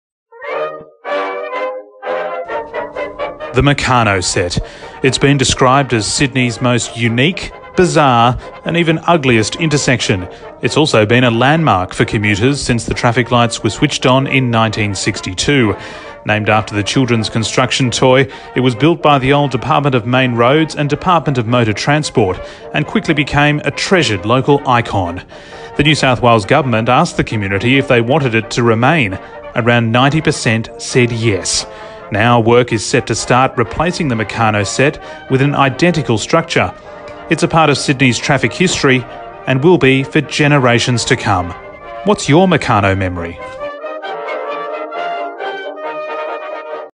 The Meccano Set - Sydney's ugliest intersection?
Video by Roads and Maritime Services (Transport for NSW), narrated by Tim Sowden, using new and archival footage.